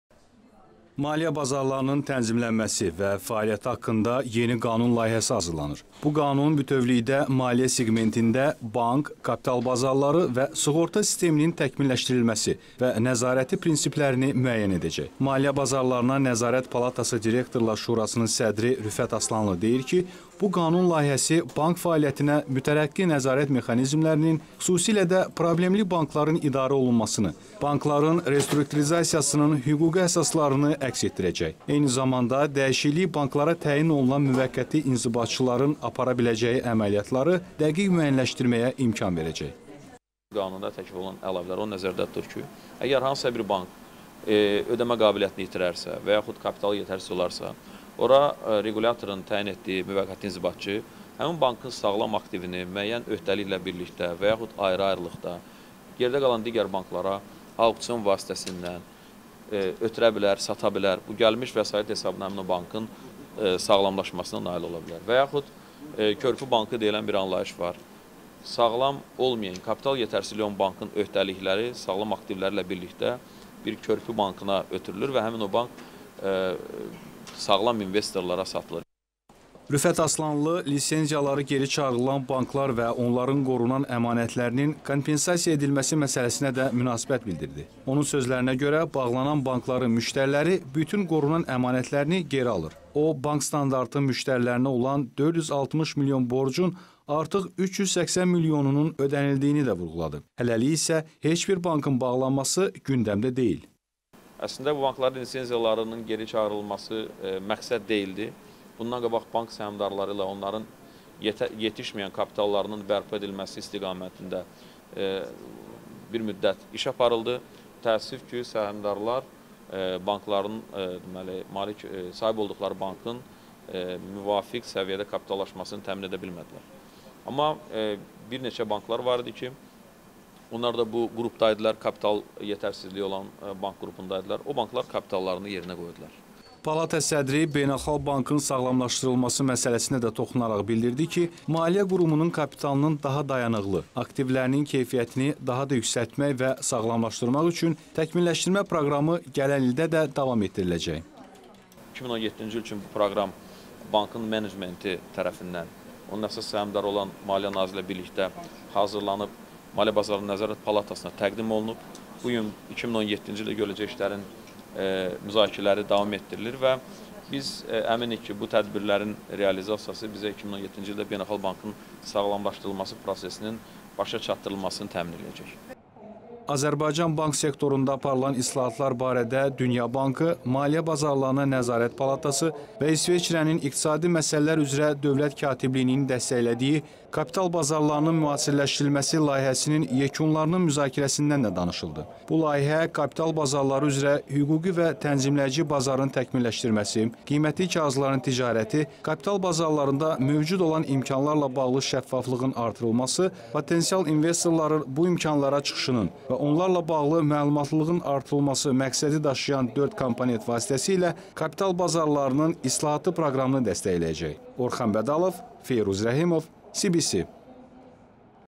Maliyyə bazarlarının tənzimlənməsi haqqında yeni qanun olacaq
"Maliyyə bazarlarının tənzimlənməsi və fəaliyyəti haqqında"  yeni qanun layihəsi  hazırlanır. Bu qanun bütövlükdə maliyyə seqmentində - bank, kapital bazarları və sığorta sisteminin təkmilləşdirilməsi və nəzarəti prinsiplərini müəyyən edəcək. Maliyə Bazarlarına Nəzarət Palatası direktorlar şurasının sədri Rüfət Aslanlı deyir ki, Bu qanun layihəsi bank fəaliyyətinə mütərəqqi nəzarət mexanizmlərinin, xüsusilə də problemli bankların idarə olunmasını, bankların restrukturizasiyanın hüquqi əsaslarını əks etdirəcək. Eyni zamanda dəyişiklik banklara təyin olunan müvəqqəti inzibatçıların apara biləcəyi əməliyyatları dəqiq müəyyənləşdirməyə imkan verəcək.

“Qanuna təklif olunan əlavələr onu nəzərdə tutur ki, əgər hər hansı bank ödəmə qabiliyyətini itirərsə və yaxud kapitalı yetərsiz olarsa, ora tənzimləyicinin təyin etdiyi müvəqqəti inzibatçı həmin bankın sağlam aktivini müəyyən öhdəliklə birlikdə və yaxud ayrı-ayrılıqda yerdə qalan digər banklara hərrac vasitəsilə ötürə, sata bilər. Gəlmiş vəsait hesabına həmin o bankın sağlamlaşmasına nail ola bilərik. Yaxud "körpü bank" adlanan bir anlayış var. Sağlam olmayan, kapital yetərsizliyi olan bankın öhdəlikləri sağlam aktivləri ilə birlikdə bir "körpü bank"a ötürülür və həmin bank sağlam investorlara satılır”.

Rüfət Aslanlı  lisenziyaları geri çağrılan banklar və onların qorunan əmanətlərinin  kompensasiya edilməsi məsələsinə də münasibət bildirdi. Onun sözlərinə görə   bağlanın  bankların  müştəriləri bütün qorunan əmanətlərini  geri alır. O, Bank Standartın müştərilərinə olan 460 milyon borcun artıq 380 milyonunun  ödənildiyini də vurğuladı. Hələlik isə heç bir bankın bağlanması gündəmdə deyil.

“Əslində, bu bankların lisenziyalarının geri çağrılması məqsəd deyildi.Bundan qabaq bank səhimdarları ilə və onların yetişməyən kapitallarının bərpa edilməsi istiqamnətində  bir müddət işlər aparıldı.Amma təəssüf ki banklar sahib olduqları bankın səhimlərini bərpa edə bilmədilər.Amma bir neçə bank var idi ki,onlar da bu qurupda idilər.Kapital yetərsizliyi olan qurupda.O banklar kapitallarını yerinə qoydular”.

Palata sədri Beynəlxalq Bankın sağlamlaşdırılması məsələsinə də toxunaraq bildiriDİ ki, maliyə qurumunun kapitalının daha dayanıqlı,aktivlərinin keyfiyyətini daha da yüksəltmək və sağlamlaşdırmaq üçün  təkmilləşdirmə proqramı gələn ildə də davam etdiriləcək.

“2017-ci il üçün bu proqram  bankın menecmenti  tərəfindən  və  onun əsas səhimdarı olan Maliyə Nazirliyi  ilə birlikdə hazırlanıb Nəzarət Palatasına təqdim edilib.Bu gün 2017-ci ildə görüləcək işlərin müzakirələri davam etdirilir.Biz əminik ki bu tədbirlərin realizasiyası  2017-ci ildə bizə  Beynəlxalq Bankın sağlamlaşdırılması məsələsini başa çatdıracaq”.

Azərbaycan bank sektorunda aparılan islahatlar barədə Dünya Bankı ,Maliyə Bazarlarına Nəzarət Palatası  və İsveçirənin İqtisadi  Məsələlər üzrə Dövlət Katibliyinin dəstəklədiyi  “Kapital Bazarlarının Müasirləşdirilməsi” layihəsinin  yekunlarının müzakirəsində də danışıldı. Bu Layihə kapital bazarları üzrə hüquqi və tənzimləyici bazanın təkmilləşdirməsi ,qiymətli kağızların ticarəti, kapital bazarlarında mövcud olan imkanlarla bağlı  şəffaflığın artırılması,potensial investorların  bu imkanlar çıxışının  və onlarla bağlı məlumatlılığın  artırılması  məqsədi daşıyan  dörd komponent  vasitəsi ilə  kapital bazarlarının islahatı proqramını dəstəkləyəcək.